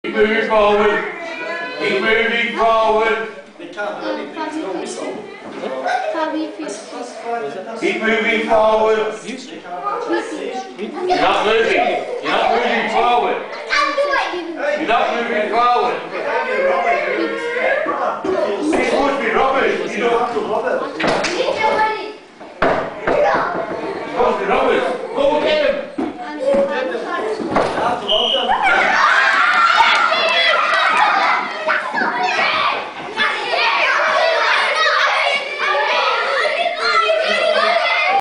Thinkers Forum Scouts - UK
This video briefly captures some indoor activities of muslim scouts children.